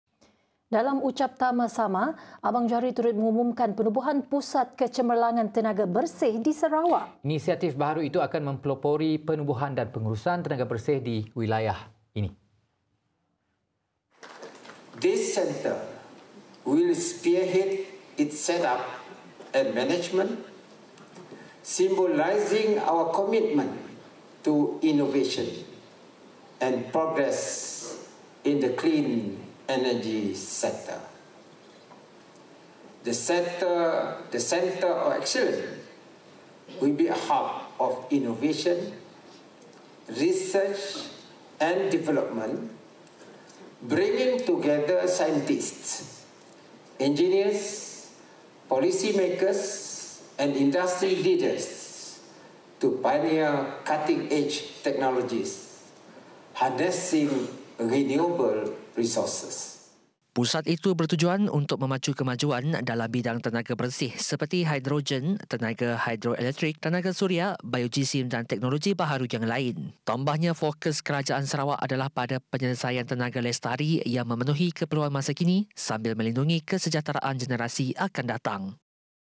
Sarawak tubuh Pusat Kecemerlangan Tenaga Bersih | TVS
Premier Sarawak, Datuk Patinggi Tan Sri Abang Johari Tun Openg mengumumkan penubuhan Pusat Kecemerlangan Tenaga Bersih di Sarawak.

Menurutnya, inisiatif baharu itu akan mempelopori penubuhan dan pengurusan tenaga bersih di wilayah ini.

📱 Strim live di tvsarawak.my atau di aplikasi MYTV Mana-Mana, Unifi TV, dan Astro GO app sekarang!